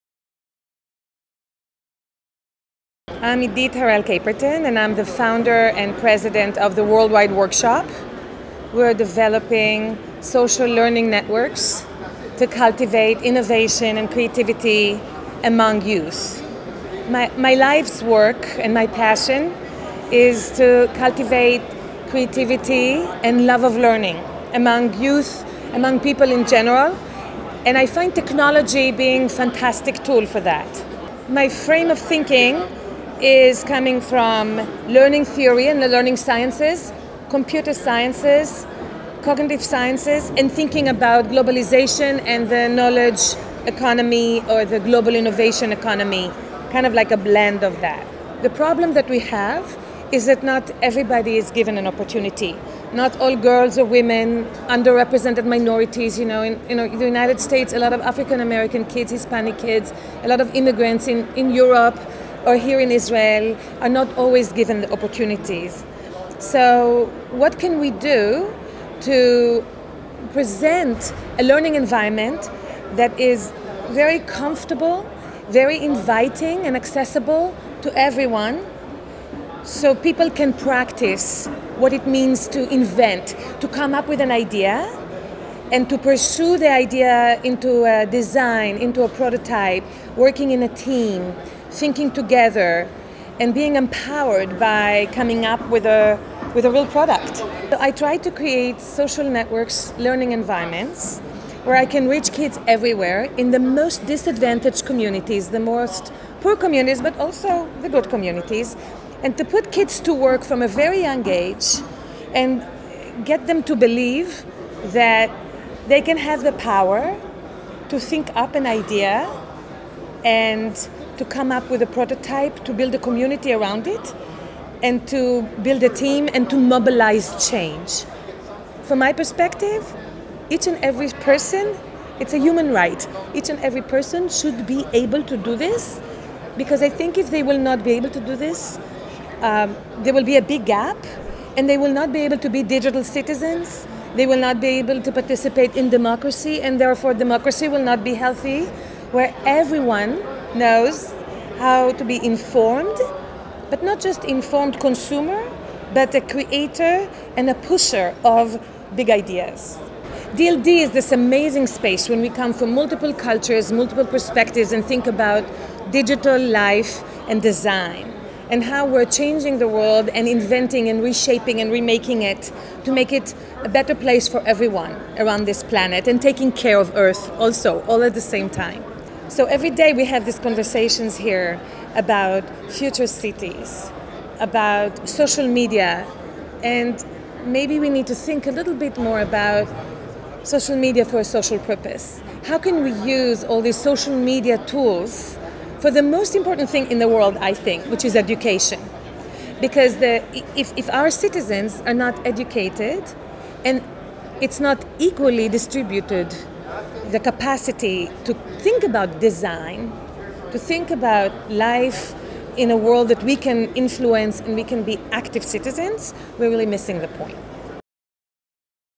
DLD Tel Aviv Interview 2011 - Idit Harel Caperton
As the founder and president of WorldWideWorkshop, Idit Harel Caperton is developing social learning networks to cultivate innovation and creativity among youth. Computer technology for her is the perfect tool to present learning environments to everyone, in particular to minorities all over the world. She in particular tries to reach kids and their parents, wherever they are based, to enable them to believe in their ideas and to pursue those ideas until they may become a prototype and in the end a product.

"It is a human right", Idit Harel at DLD Tel Aviv argues, "to be informed not only as a consumer, but also as a creator and pusher of one`s ideas". So as one of the programs of her organisation, she installed the Globaloria Game Design Competitions, a social learning network, teaching students to invent educational webgames. "To be digital citizens in democratic societies, we must be aware, that our most important commodity is education", she summarizes her mission. Meet Idit Harel in the video below to also discuss, why social media should be better used for social purposes.